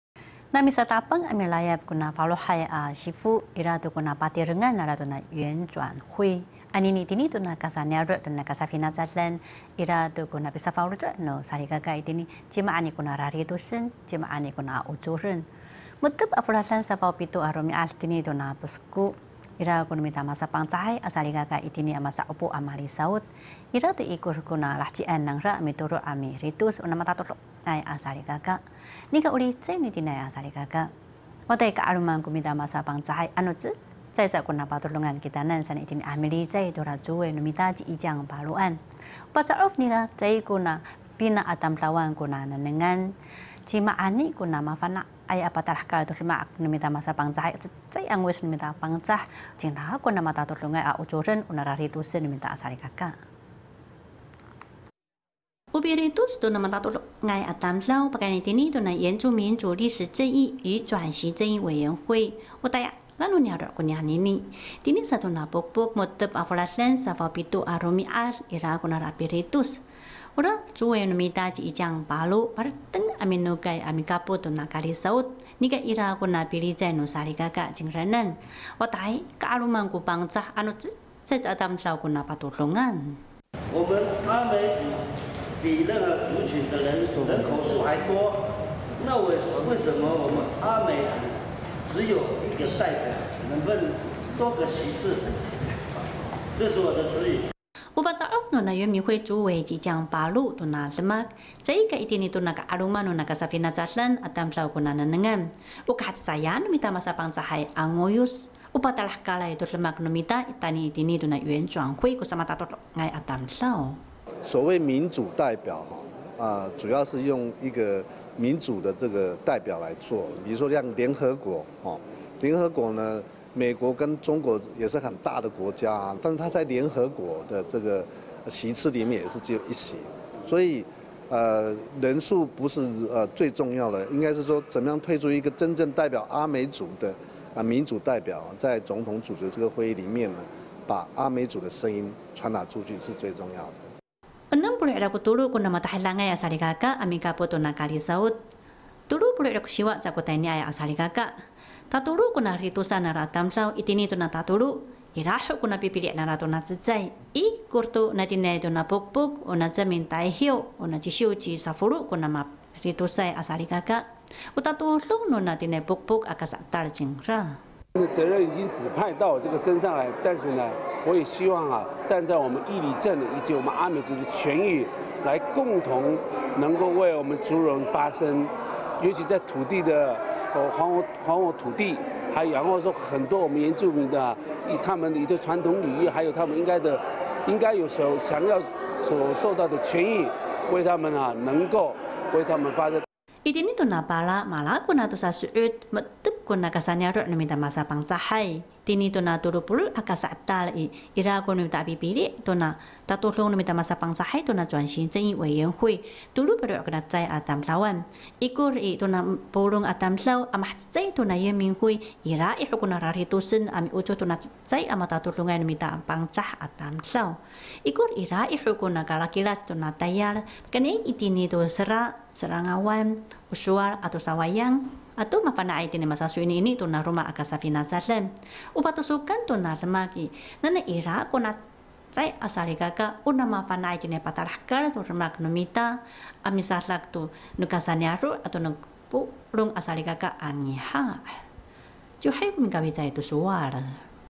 原轉正阿美族代表 玉里人選出爐 2016-10-20 Amis TITV 原視族語新聞
O piditos no kasa'etal to matatodongay a ocoden

設置在總統府下的原轉會，各個族群的代表已經陸陸續續的推派出來，10月17日在花蓮玉里Posko舉辦的阿美族族群代表推舉會，就有與會的人員表示，因為阿美族人口多的關係，不應該只有一個席次的代表。不過原民會主委也做出回應，人數的多寡並不是重點，重要的是，要把部落的聲音確實的傳達出去。

各原住民族群針對原住民族歷史正義與轉型正義委員會，陸續推出族群代表，花蓮縣玉里鎮10月17日舉辦阿美族群代表推舉會議，原民會主委夷將‧拔路兒也回到自己故鄉參加這場會議，會議中有族人提到，阿美族人數多，不應該只有1席。

原民會主委夷將‧拔路兒再次表示，人數代表多寡並非最重要，而是能否將族群聲音在原轉會中正確傳達才是最重要。

這場推舉會議有63位受邀，實際到場參與有39位，會中推舉3位後，從中再表決。最後現任玉里鎮民代表的秀吉‧撒夫洛，為玉里鎮阿美族代表。

阿美族全台有210個部落，從中再選出30個分區代表，阿美族轉型正義委員會預計產生31人，再前往原民會共同推舉出1位阿美族群代表。阿美族轉正會日後會依任務編組分為土地、文化、語言、歷史及和解小組，目的是要讓族群代表更能正確傳達各地阿美族心聲。